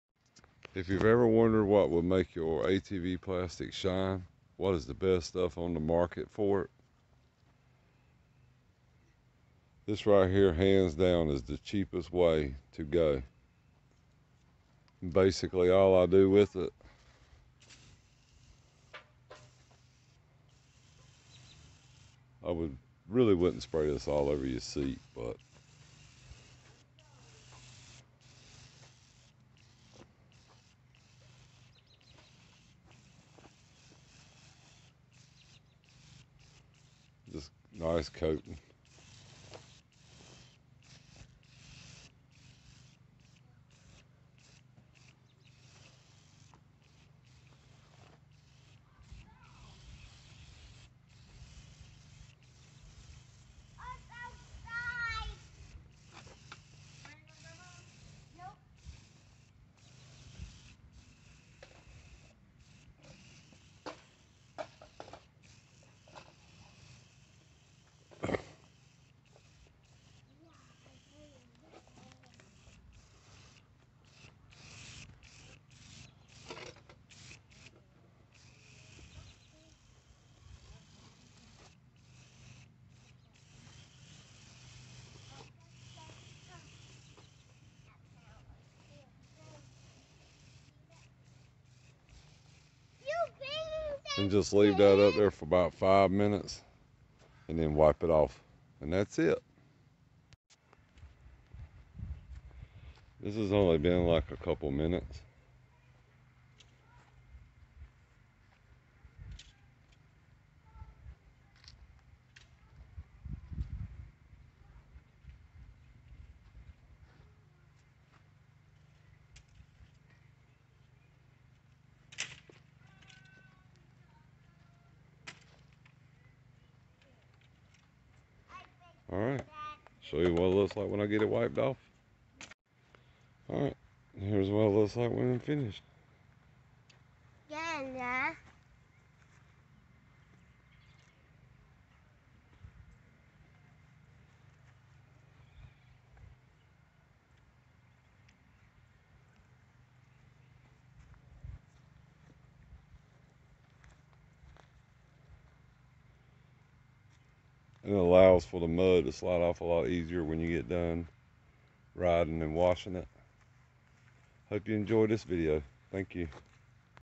How to make mud easier to get off your atv.
this is hands down and the cheapest way to make your atv shine.